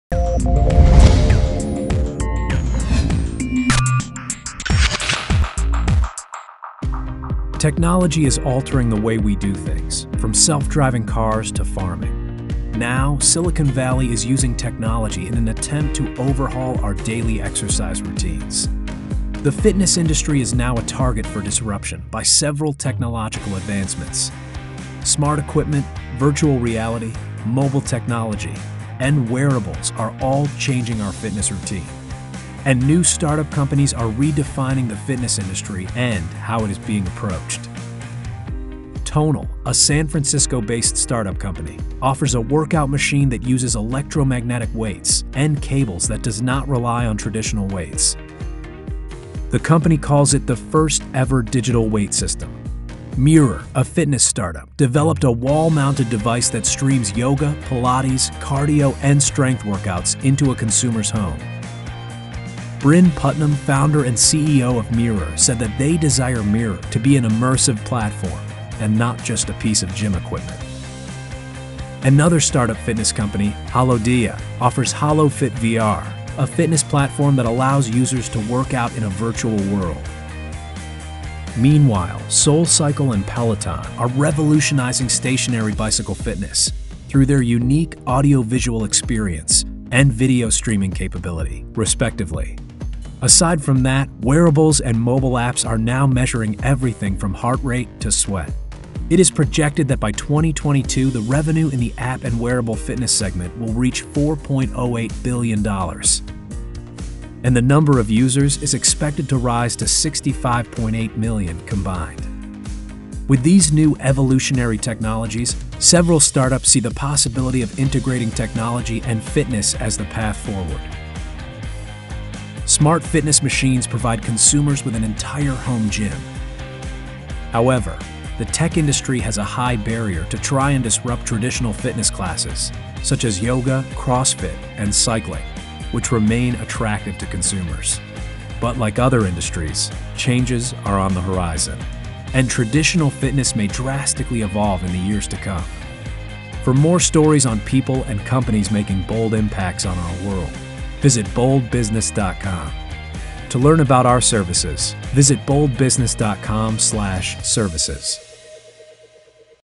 Impact of Technology on Fitness Industry | Digital Disruption
Learn how the fitness industry has evolved with digital disruption & advancements